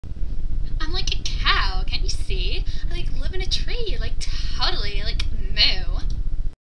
I'm like, a cow, totally
I'm a valley girl who wants to be a cow.
I have no idea what this is about. And don't tell me I was a loser, I'm already fully aware of that.
Luckily, this was 5 years ago, and I promise, I'm a lot more normal now.